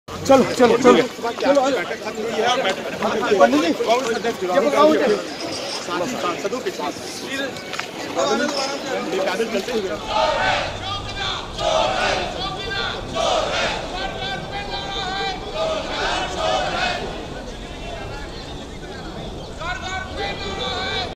اپوزیشن جماعتوں کی عظیم ریلی
سولہویں لوک سبھا کے آخری اجلاس کے پیش نظر کانگریس، ٹی ایم سی، تلگودیشم پارٹی اور دیگر اپوزیشن جماعتوں کی جانب سے احتجاجی ریلی نکالی جارہی ہے۔ یہ ریلی پارلیمنٹ سے جنتر منتر تک نکالی جارہی ہے۔اس ریلی میں ترنمول کانگریس پارٹی کے ممبران پارلیمنٹ سیاہ لباس میں پہنے ہوئے شریک تھے۔